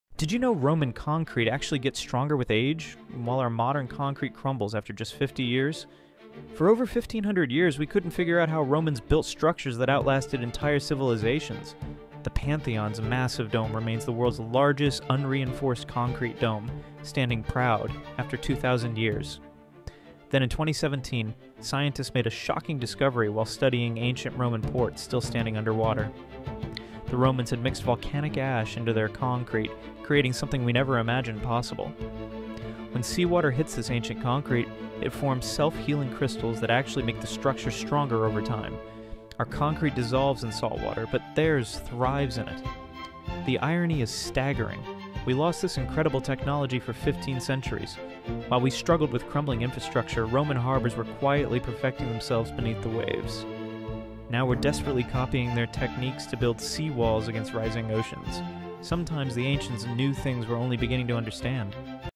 Roman Concrete Gets STRONGER With Age (Ours Crumbles in 50 Years)
Romans knew something we forgot for 1500 years